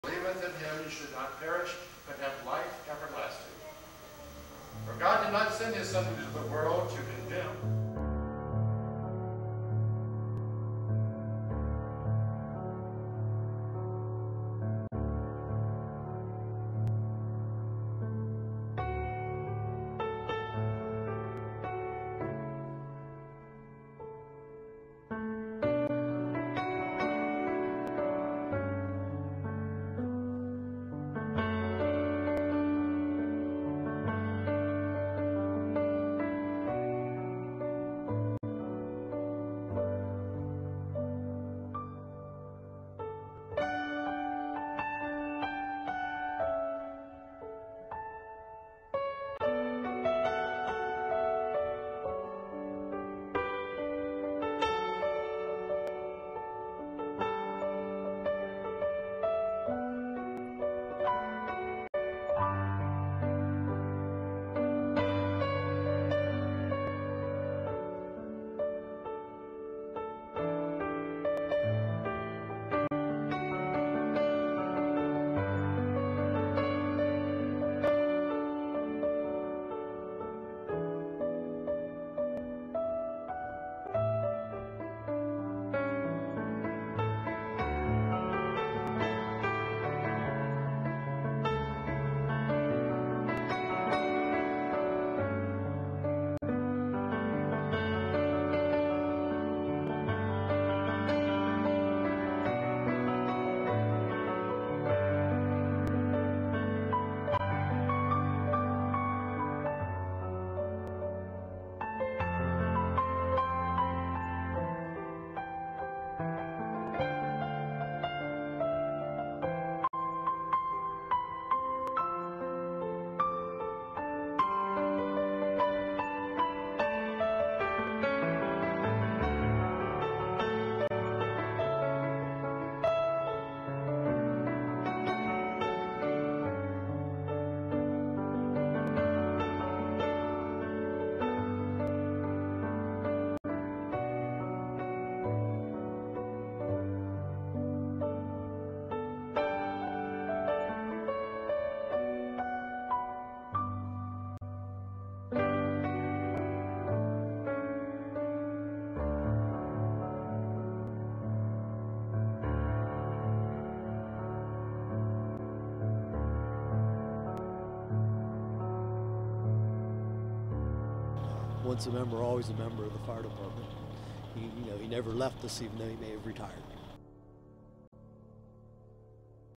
Funeral for Former Fire Chief Richard Cole
This is a short video I put together for a family after they lost a loved one.